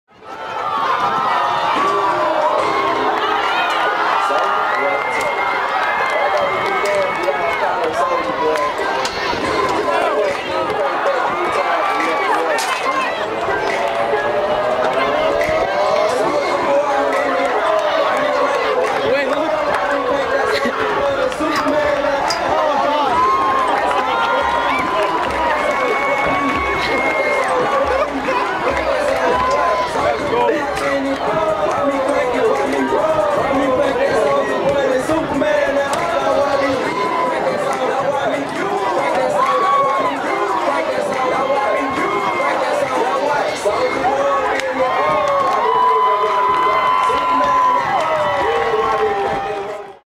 Avalanche's Soulja Boyz
Kutztown University's mascot Avalanche celebrated his birthday with his friends.  As a treat for the fans they danced to Soulja Boy.